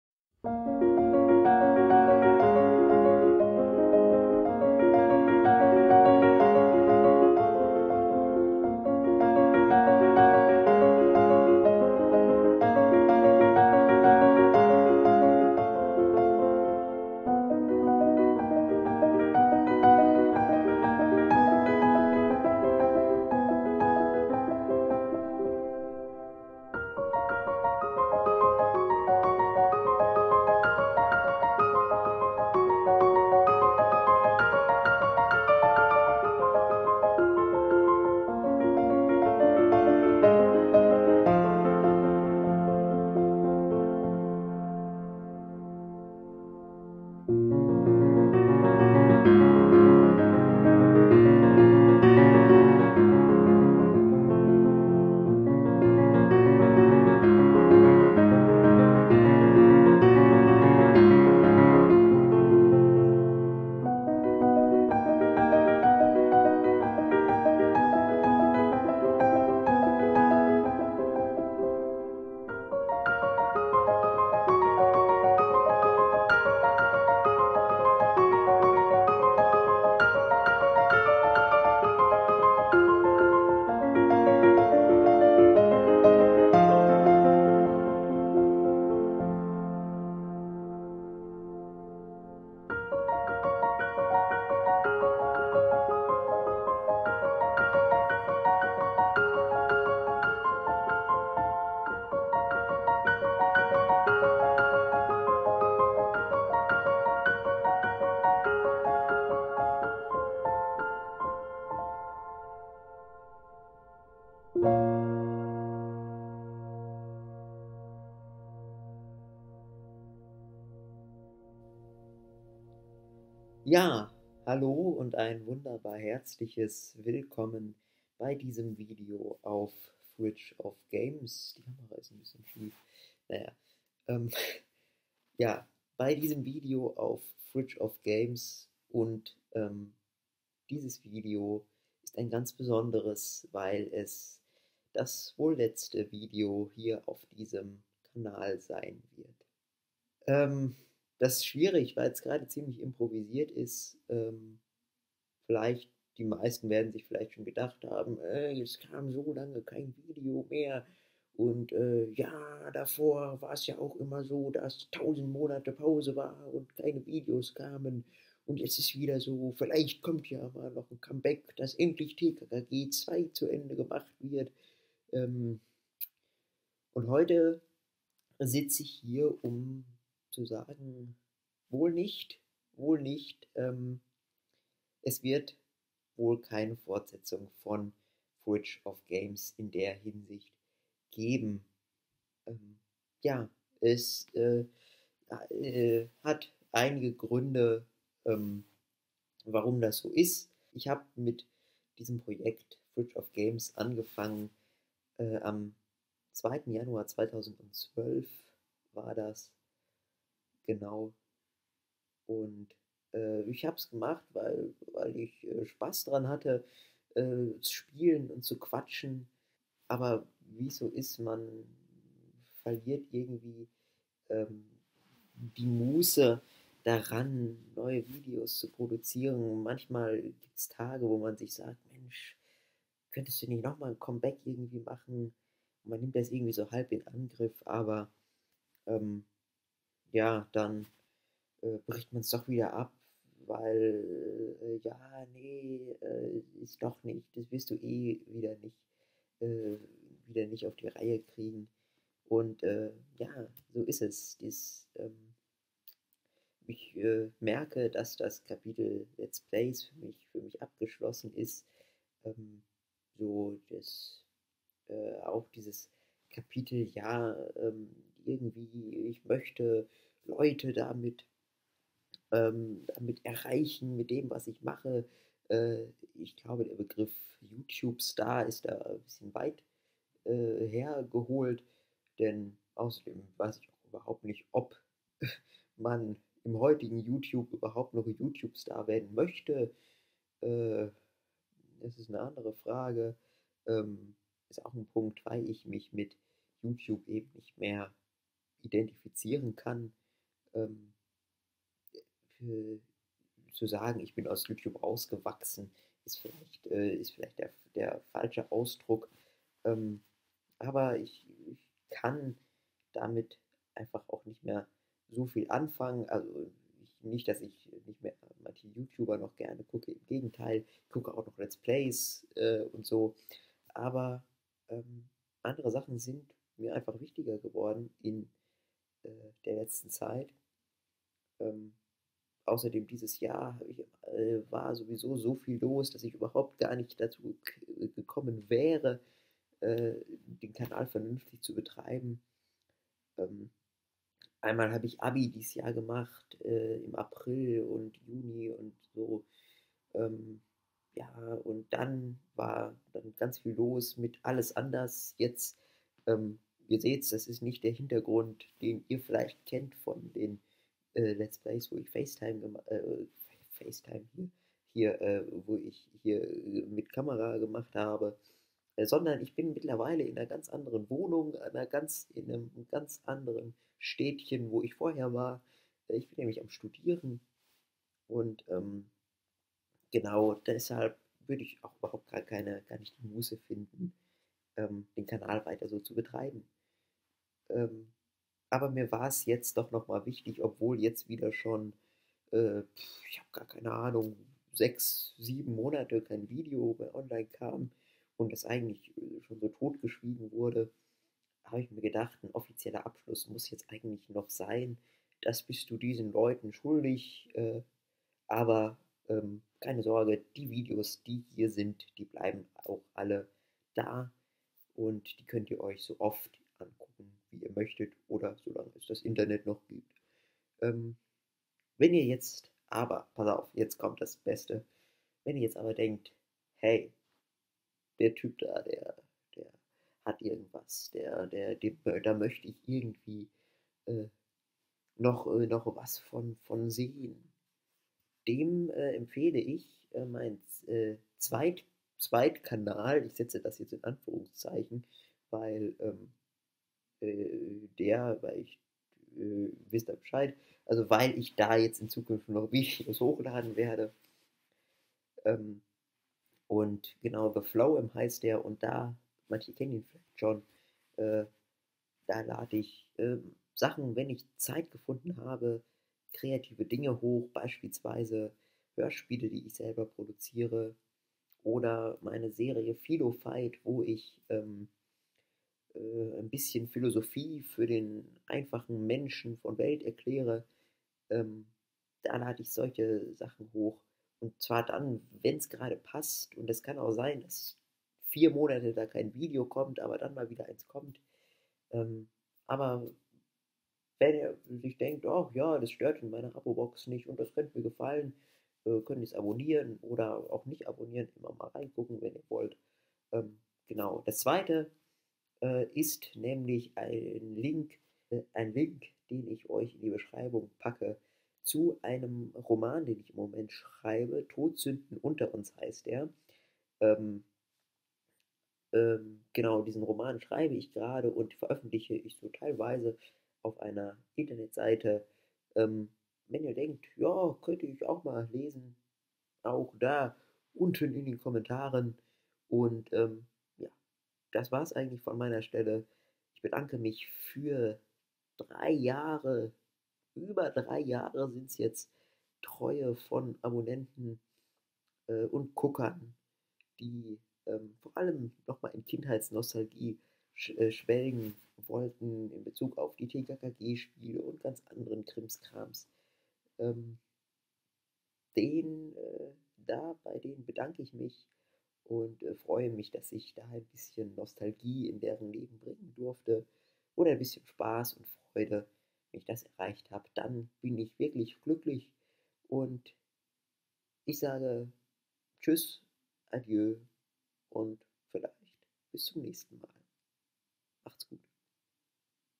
Tschüß, Adieu (und bis zum nächsten Mal?)
Vielen Dank, ihr tollen Menschen. Auch wenn das mit dem 'Grabgemetzel 3' Let's Play jetzt nichts mehr geworden ist.